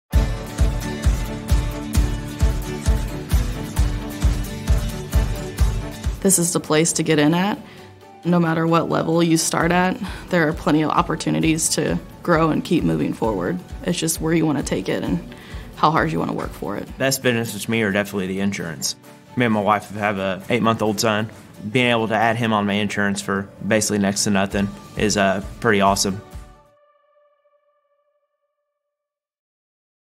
MasterBrand Sioux Falls June
Tired of working weekends? We want YOU! StarMark, a division of MasterBrand is hiring at its Sioux Falls facility. You’ll work 1st shift, Monday-Thursday, (6am-4:30pm) with overtime available on Fridays. Great benefits include medical, dental, vision, life insurance, immediate 401k eligibility, company-paid short-term disability, and opportunity for growth. Find easy applications and same-day hiring at masterbrandsiouxfalls.com.